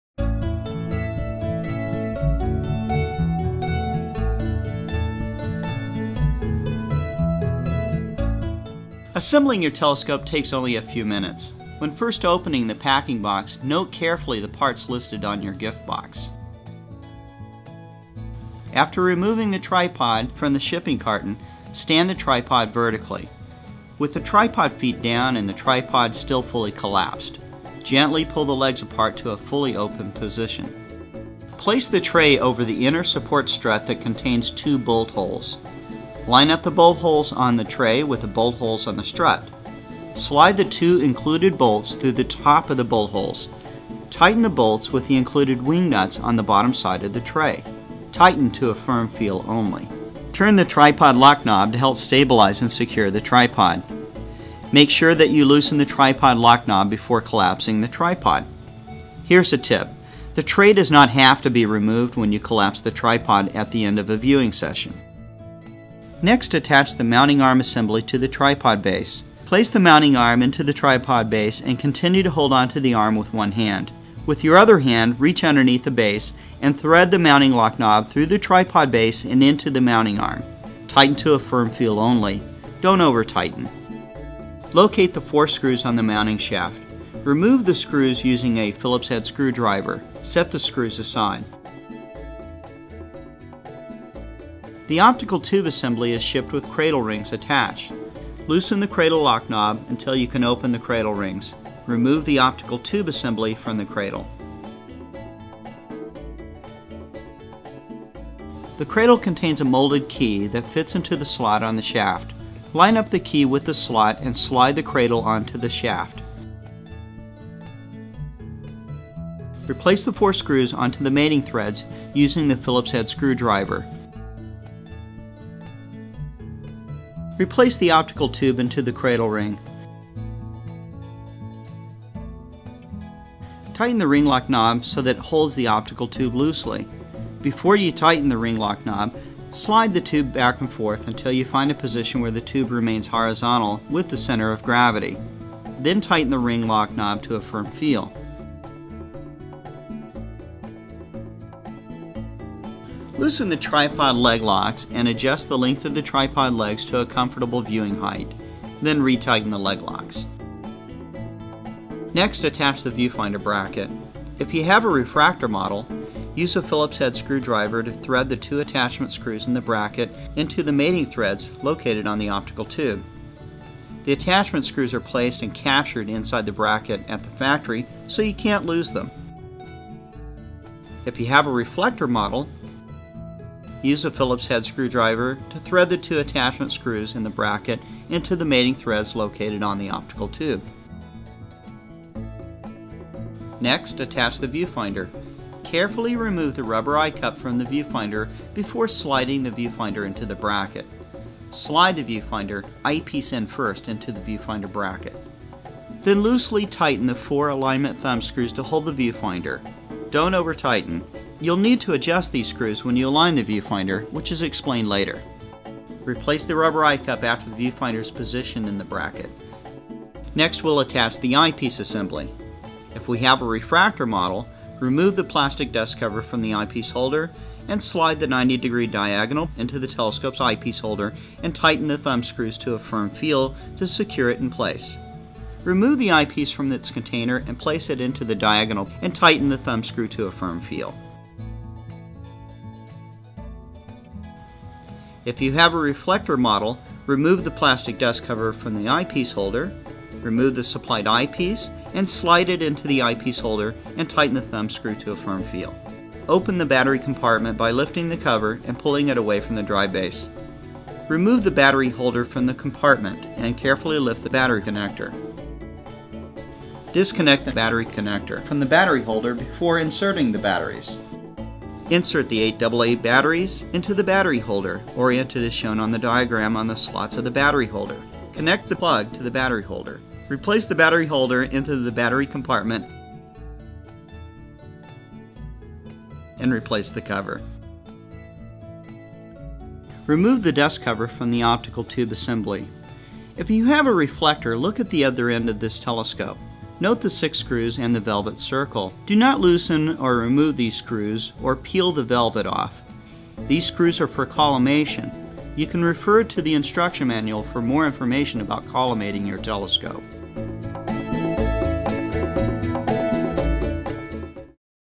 Meade Instruments How to Use Your Telescope Part 3 22 YouTube
An introductory video for anyone who uses a Meade DS series telescope. While the material is a bit dated, those of us who still use our DS-2150 Autostar Telescopes will find this useful.